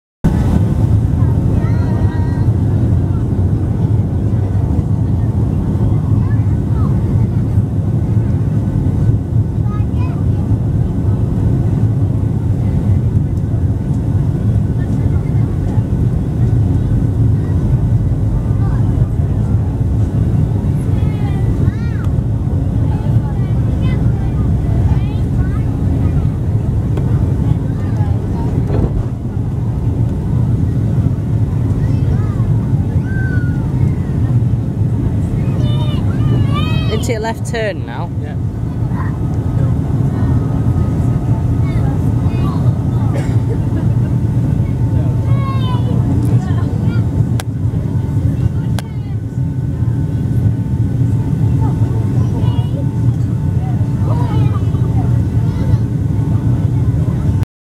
Taking off from Mahon airport onboard Thomas Cook Boeing 757-200 5/9/14
Taking off from Mahon Airport onboard Thomas Cook Boeing 757-200
flight 5:00pm Mahon to Birmingham